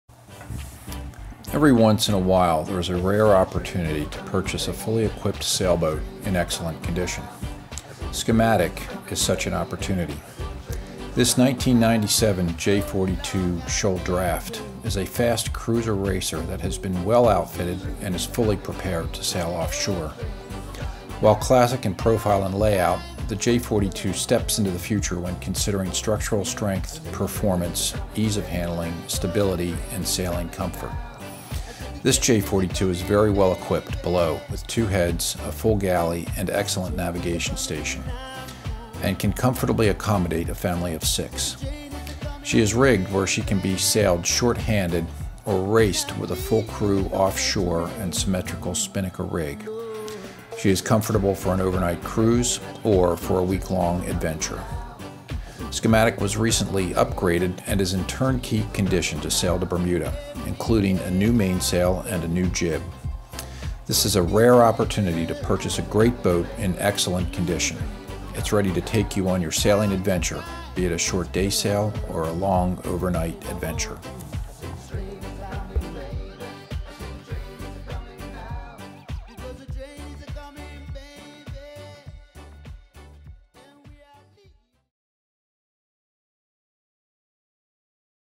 J42 Schematic for Sale
This video is about the J42 s/v Schematic which is currently for sale in Annapolis, MD